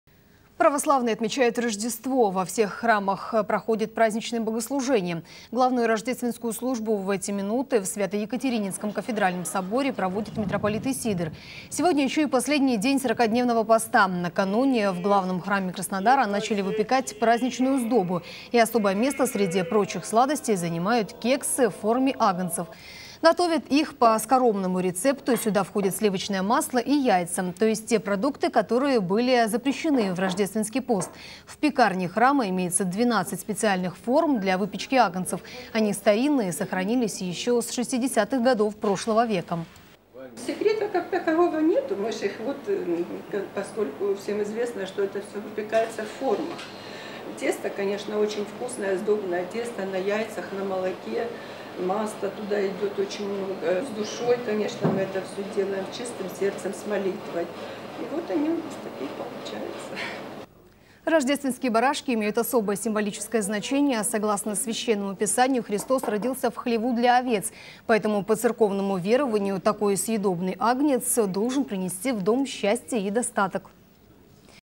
В соборах Краснодара выпекли рождественскую сдобу
Православные готовятся отметить Рождество. Сегодня сочельник и праздничные богослужения уже вовсю идут во всех храмах города.